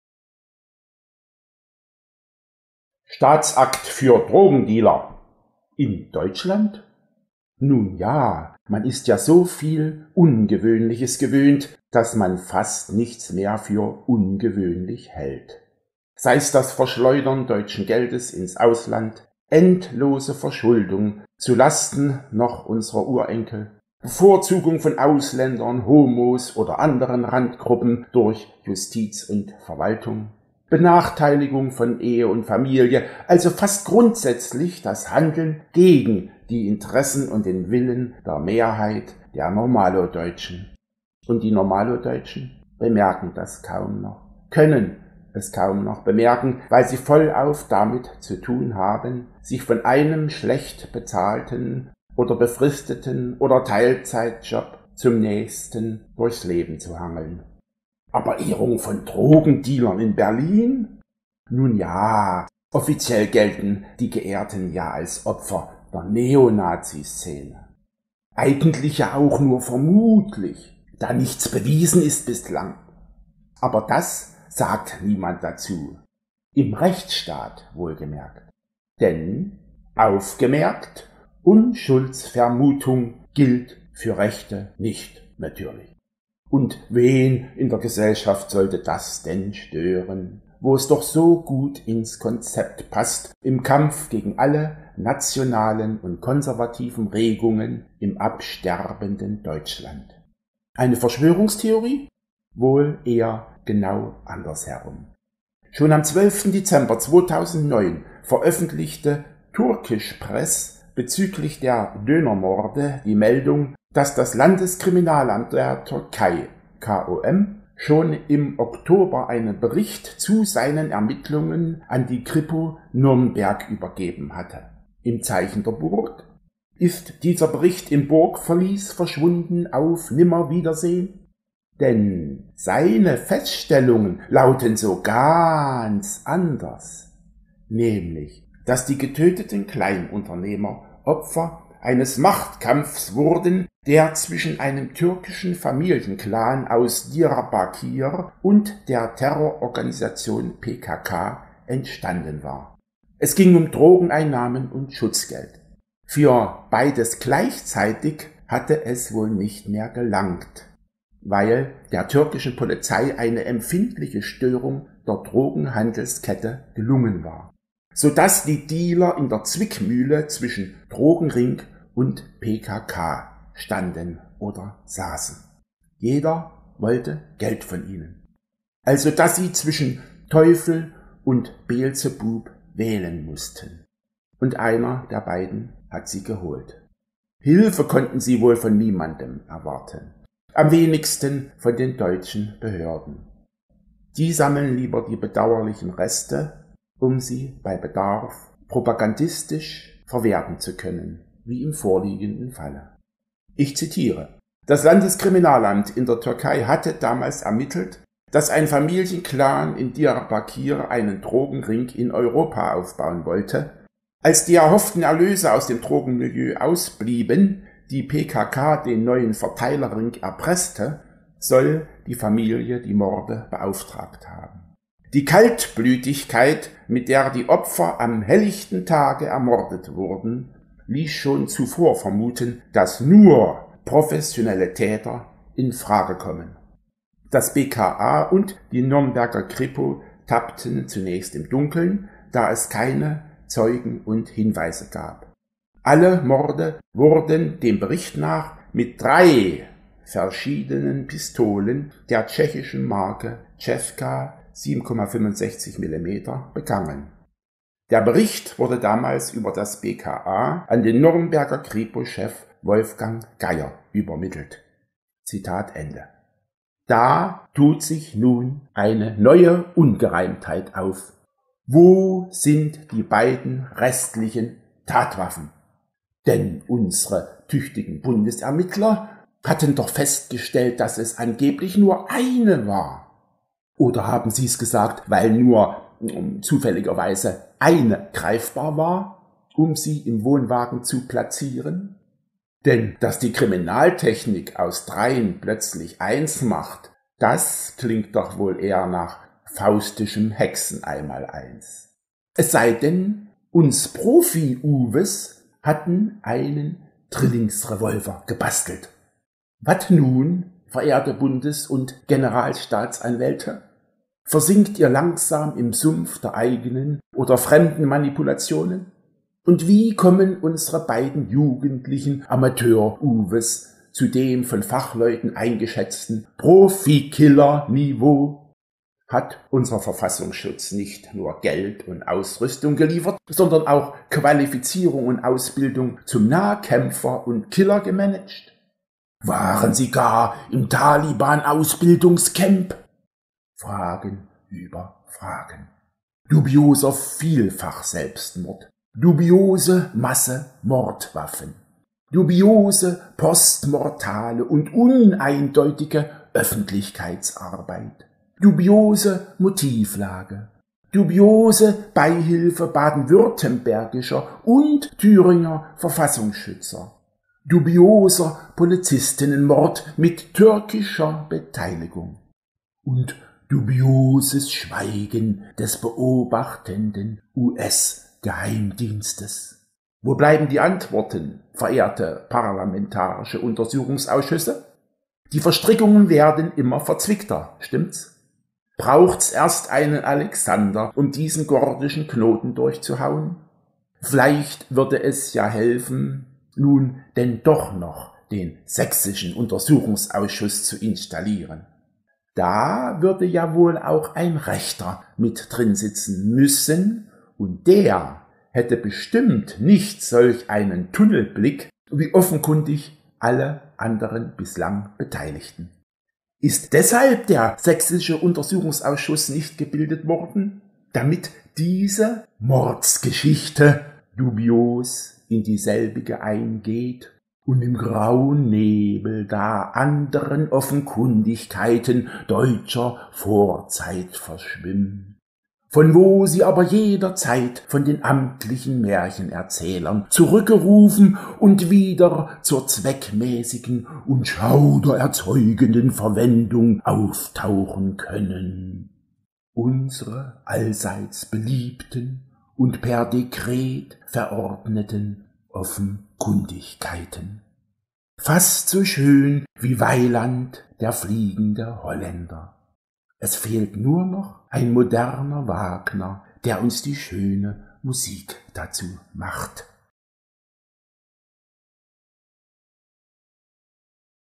Kolumne - Staatsakt für Drogendealer
Die Quersumme -- Kolumne von Hans Püschel
In Deutschland? Nun ja, man ist ja soviel Ungewöhnliches gewöhnt, daß man fast nichts mehr für ungewöhnlich hält. Sei's das Verschleudern deutschen Geldes ins Ausland, Verschuldung unserer Urenkel, Bevorzugung von Ausländern, Homos oder anderen Randgruppen durch Justiz und Verwaltung, Benachteiligung von Ehe und Familie, also fast grundsätzlich das Handeln gegen die Interessen und den Willen der Mehrheit der Normalo-Deutschen. Und der -- bemerkt das noch kaum, weil er vollauf damit zu tun hat, sich von einem schlecht bezahlten befristeten oder Teilzeitjob zum nächsten durchs Leben zu hangeln...